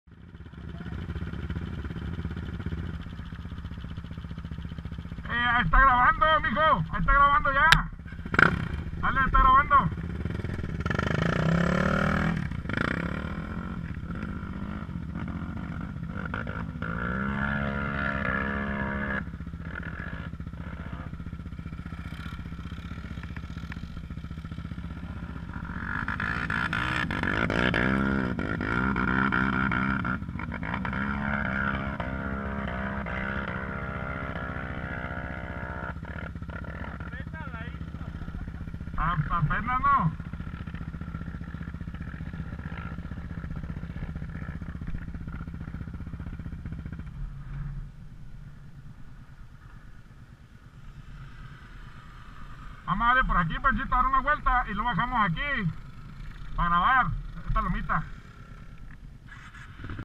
kfx 700 raptor 700 honda 700xx peach valley montrose colorado. gopro.
calando la go pro hero 3+ con las cuatrimotos peach valley.